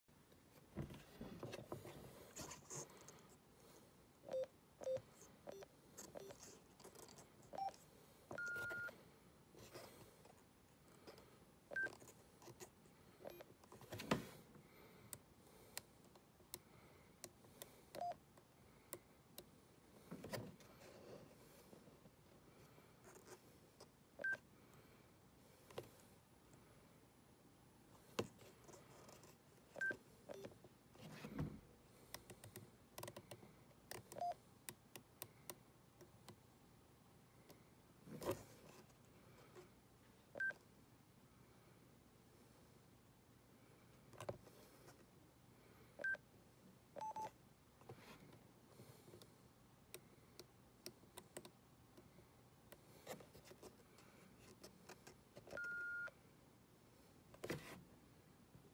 Storing a VFO into Memory for a Yaesu FT-60R
Set a working VFO and storing it into memory on a Yaesu FT-60R